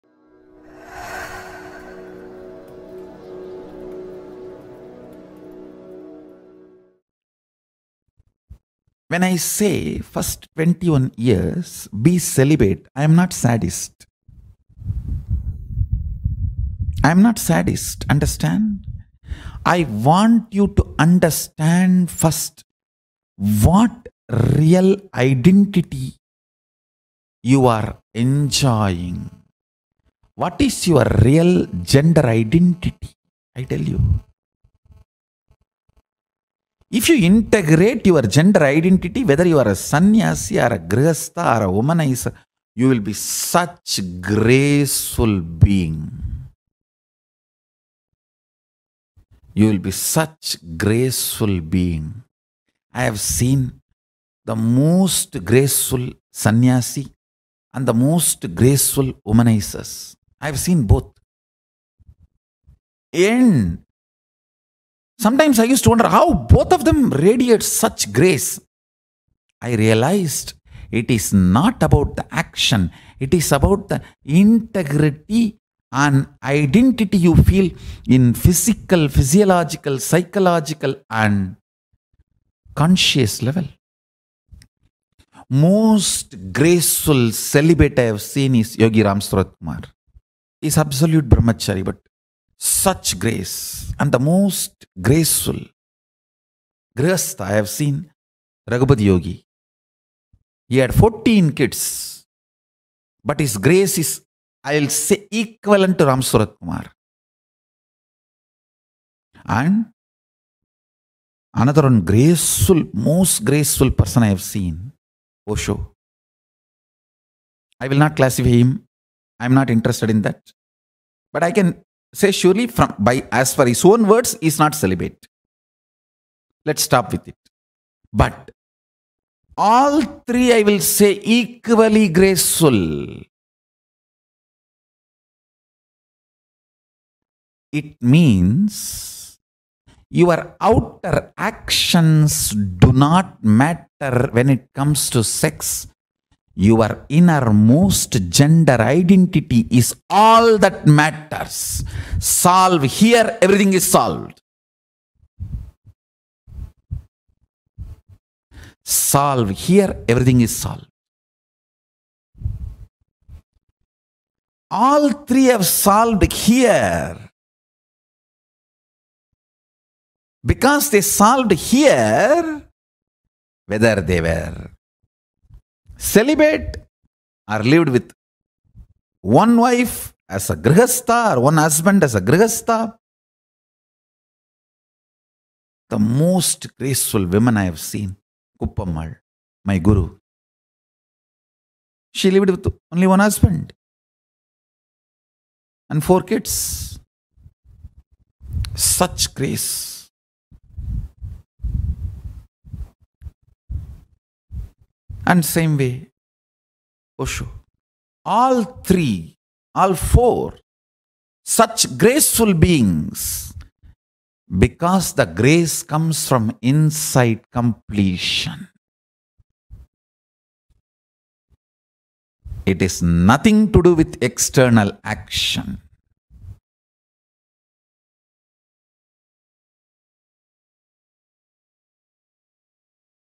Understand Why Celibacy is NOT Sadism Find Out Your Gender!
When I say, first 21 years be celibate, I am not sadist. I am not sadist. Understand, I want you to understand first what real identity you are enjoying. What is your real gender identity? I’ll tell you, if you integrate your gender identity, whether you are a sanyasi or a grehasta or a womanizer you will be such graceful being! You will be such graceful being! I have seen the most graceful Sanyasi and the most graceful womanizers, I have seen both, and, sometimes I used to wonder , how both of them radiate such grace. I realized, it is not about action, it is about the integrity and identity you feel in physical, physiological, psychological and conscious level. Most graceful celibate I have seen is Yogi Ramsurat Kumar . He is absolute Brahmachari but such grace, and the most graceful grehasta  I have seen: Raghupathi Yogi . He had 14 kids, but his grace is, i will say, equivalent to Ramsurat Kumar, and , another graceful, most graceful person I have seen, Osho. I will not classify him, I am not interested in that, but I can say surely, from, by, as for his own words, he is not celibate. Let us stop with it. But, all 3 I will say equally graceful. It means your outer actions do not matter when it comes to sex, your innermost gender identity is all that matters. Solve here, everything is solved! Solve here, everything is solved! All 3 have solved here, because they solved here , whether they were celibate, or lived with one wife as a grehasta, one husband as a grehasta, the most graceful woman I have seen Kuppamal,  my guru. She lived with only one husband and 4 kids. Such grace! And, same way, Osho, all 3, all 4, such graceful beings, because the grace comes from inside completion. It is nothing to do with external action.